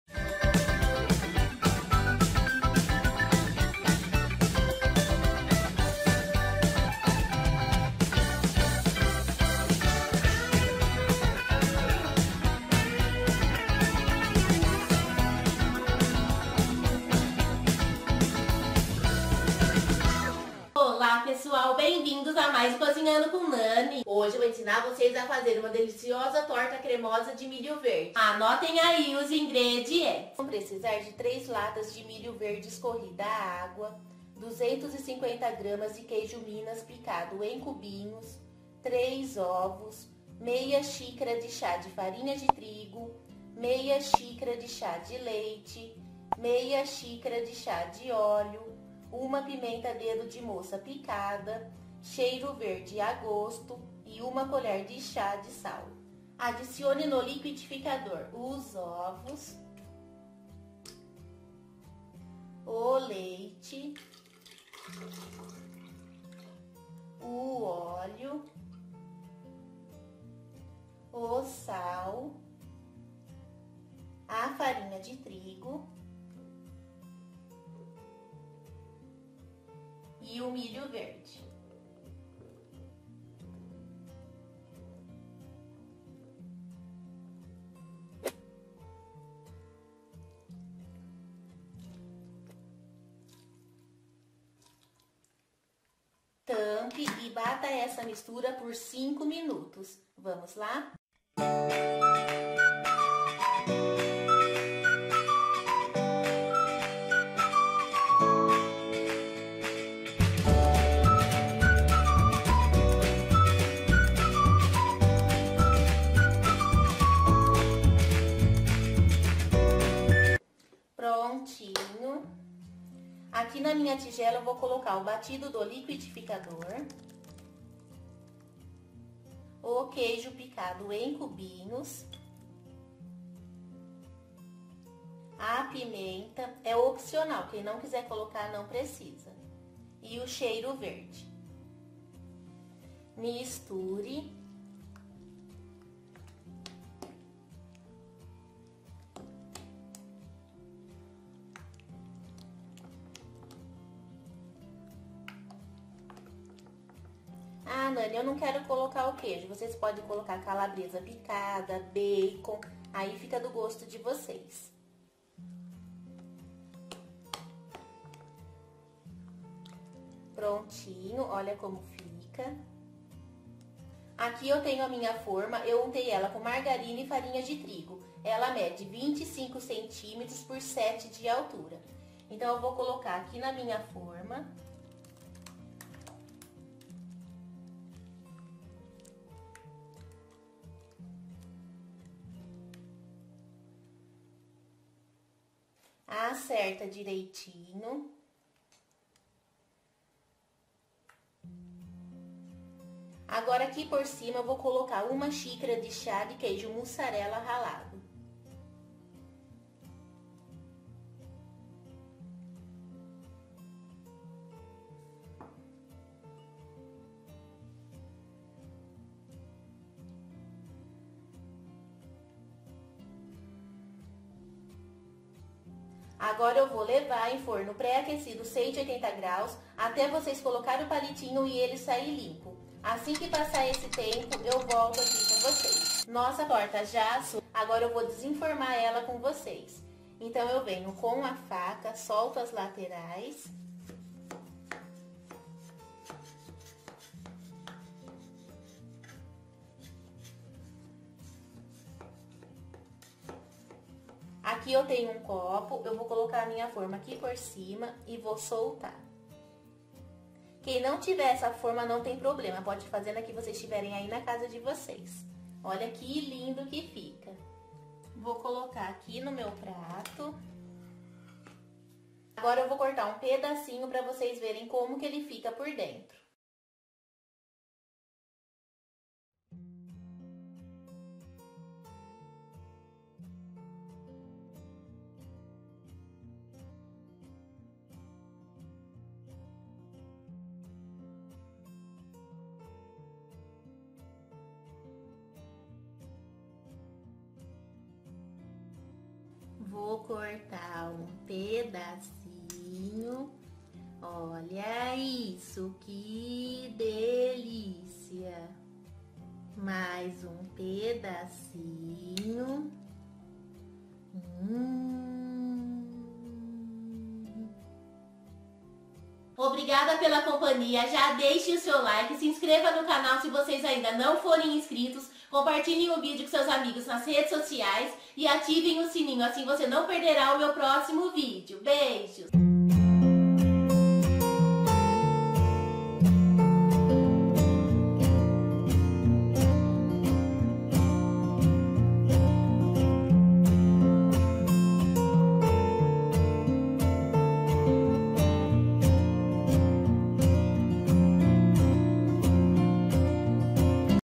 TORTA DE MILHO DE LIQUIDIFICADOR - FÁCIL E DELICIOSA com CozinhandoComNane :)
Hoje eu vou ensinar a fazer uma deliciosa TORTA SALGADA DE MILHO VERDE !!! Essa torta fica tão cremosa e saborosa que vcs irão se surpreender !!! Além de ser bem rápido e fácil  de preparar !!!

Já deixe aquele 👍para ajudar o canal e comente o que vcs acharam !!! Beijinhos !!!

🔴ANUNCIE AQUI NO CANAL COZINHANDOCOMNANE

❤ Os ingredientes são:

3 latas de milho verde
250 gramas de queijo minas picado em cubinhos
3 ovos
1/2 xícara de chá de farinha de trigo sem fermento
1/2 xícara de chá de leite
1/2 xícara de chá de óleo
1 pimenta dedo de moça picada sem semente
cheiro verde a gosto
1 colher de chá de sal

Xícara utilizada: 240 ml

Assadeira de 25 cm por 7 de altura